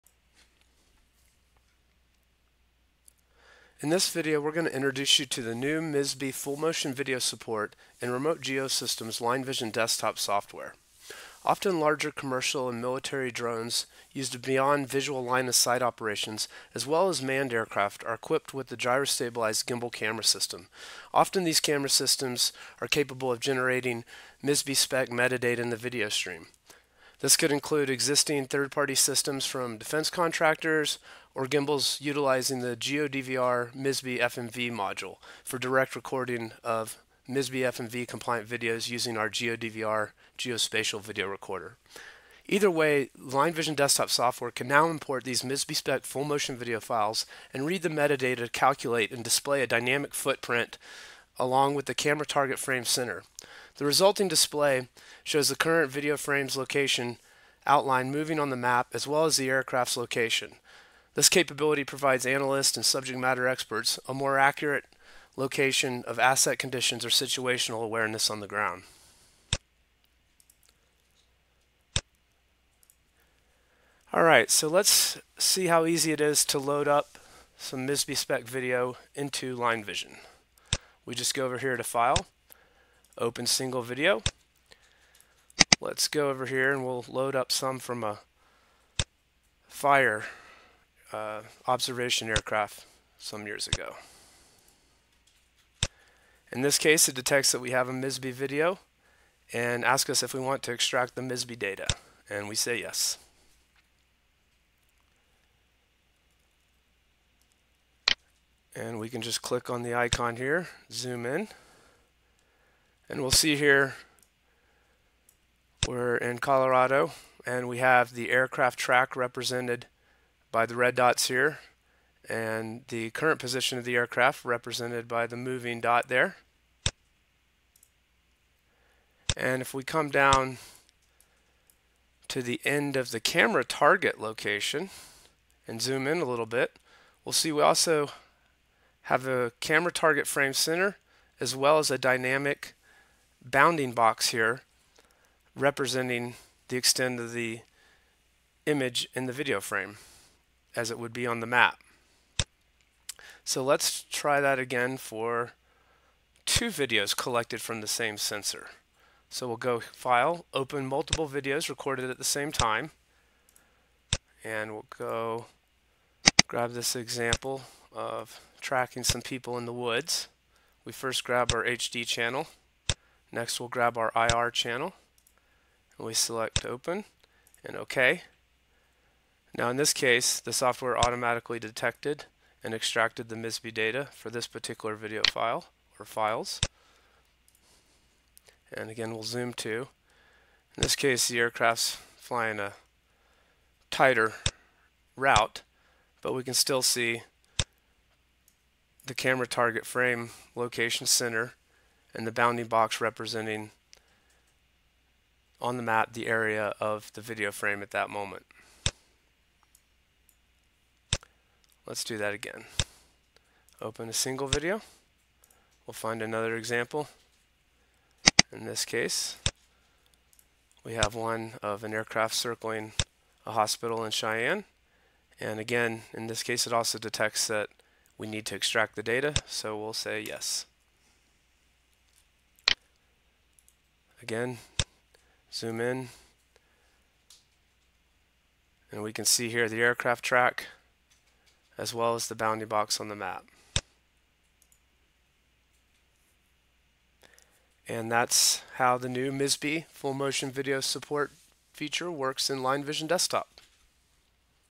Intro to MISB Full Motion Video (FMV) Playback in LineVision Desktop
Introduction to using Remote GeoSystems LineVision™ Desktop suite to playback and analyze MISB-spec Full Motion Video (FMV).  In addition to "platform-only geotagged" video files, LineVision Desktop now supports map-based playback and geoProject™ reporting with MISB FMV-compliant (STANAG 4609) videos to display a dynamic “camera target footprint” on top of Esri-based maps.